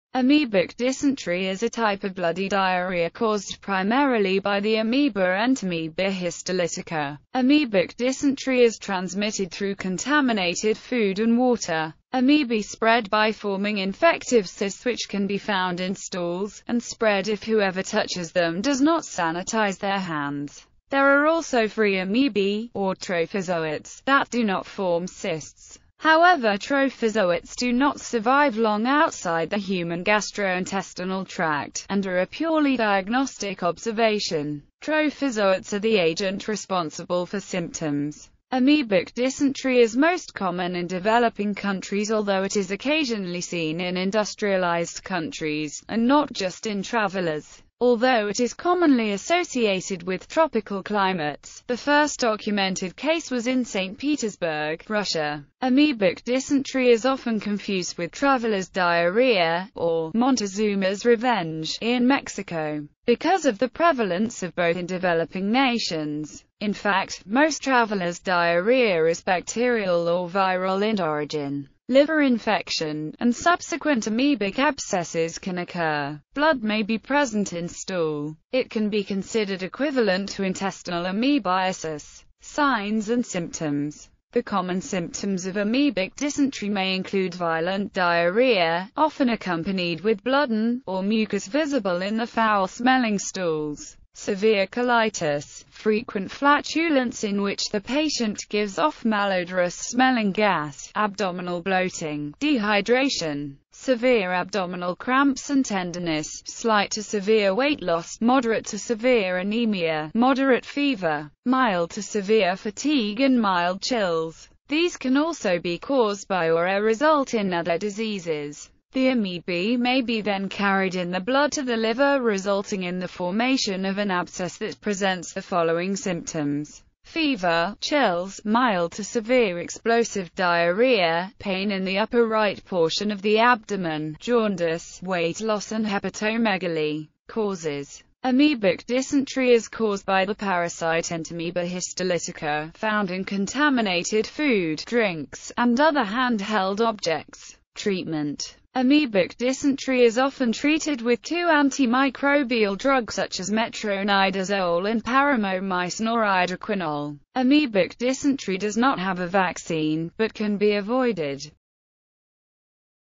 Amoebic dysentery
Amoebic dysentery (or amebic dysentery) is a type of bloody diarrhea caused primarily by the amoeba Entamoeba histolytica.Amoebic dysentery is transmitted through contaminated food and water.